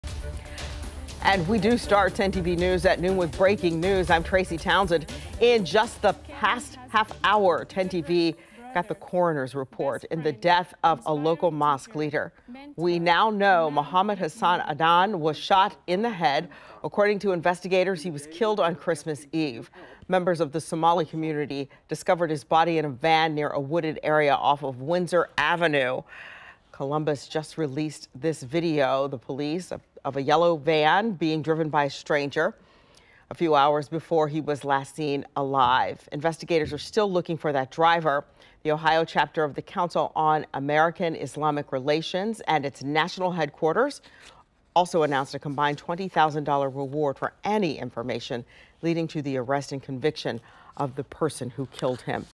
Autopsy report say Columbus mosque leader was shot multiple times
Mohamed Adam was found in a van near a wooded area off of Windsor Avenue, west of Joyce Avenue on Dec. 24.